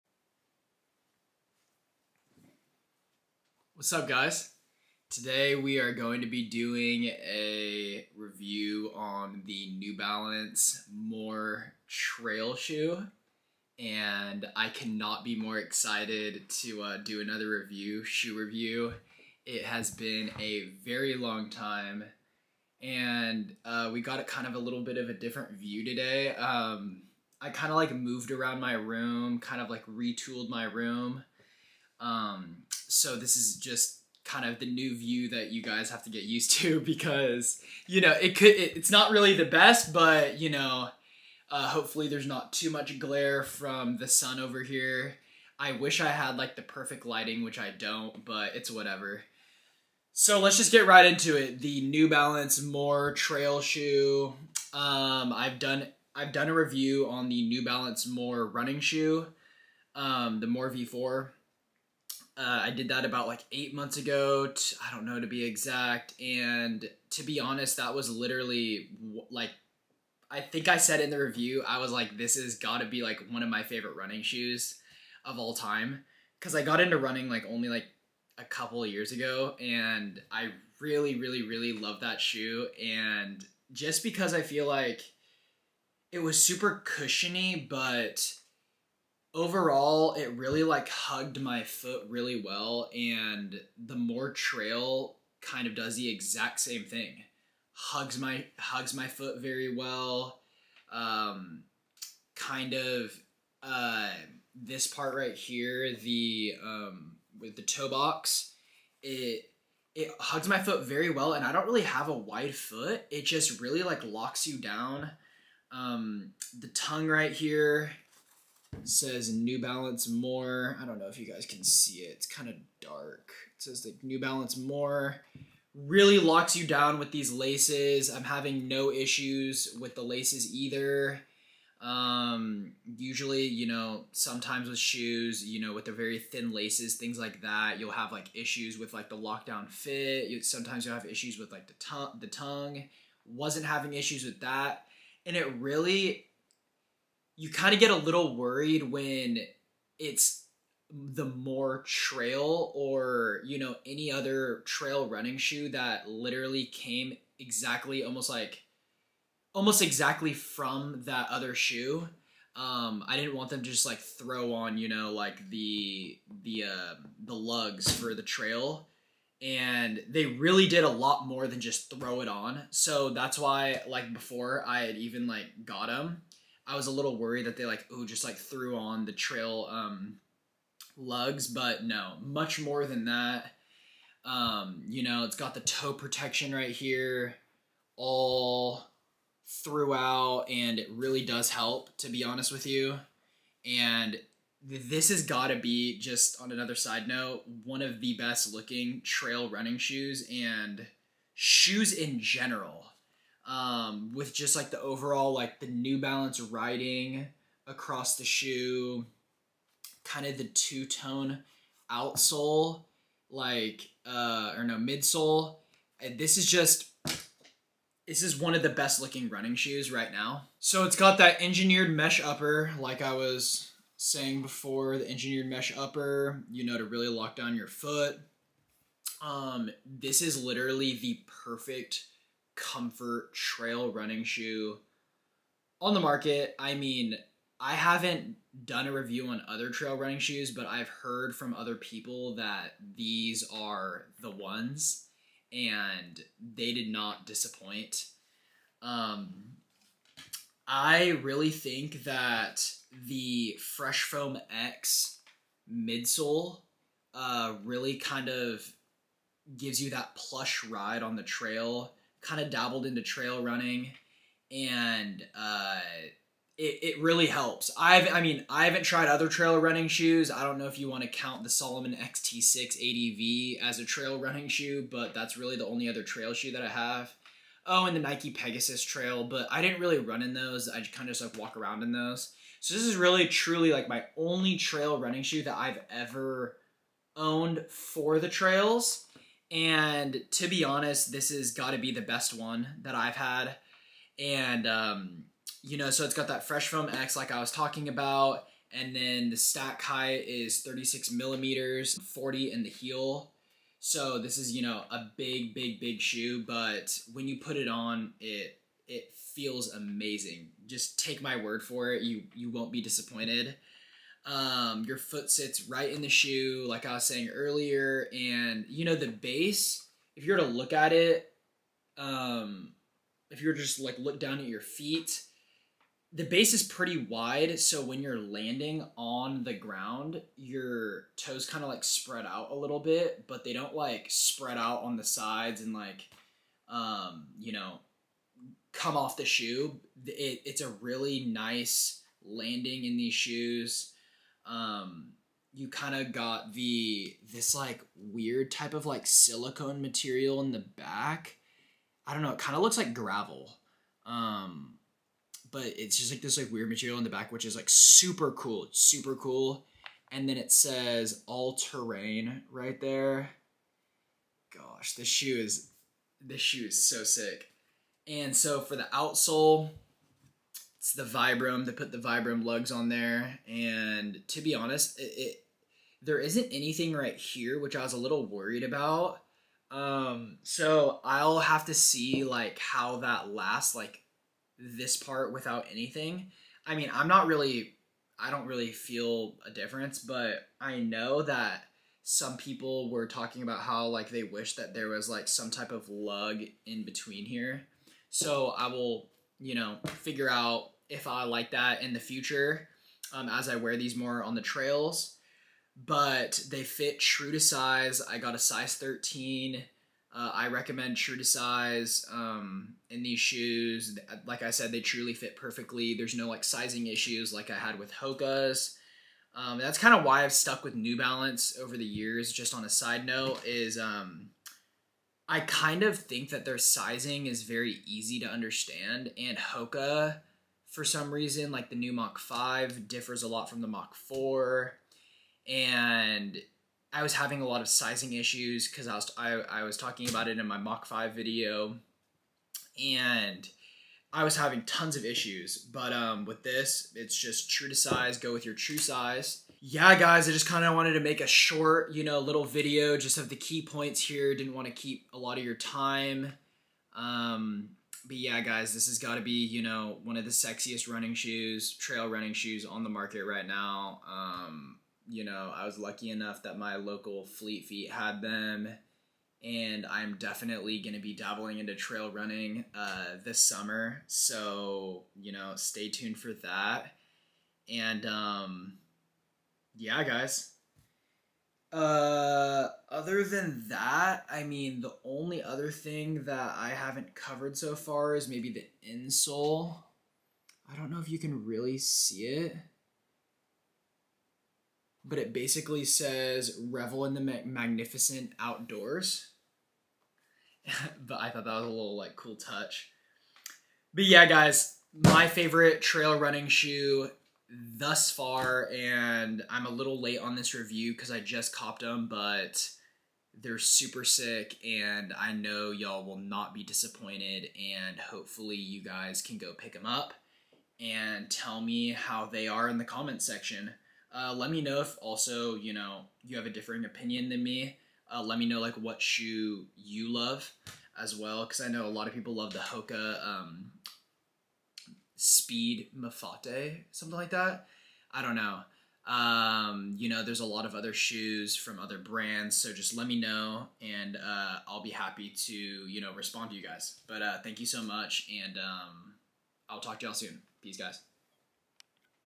New Balance More Trail v3 Shoe Review | Is this the best trail running shoe?
Hey whats up guys hopefully everything has been going well.  I recently got a pair of New Balance More v3 Trail shoes so I thought it would be a great opportunity to give ya'll my honest review.  I really loved these shoes because they were so responsive and comfort driven.  Leave some comments down below!!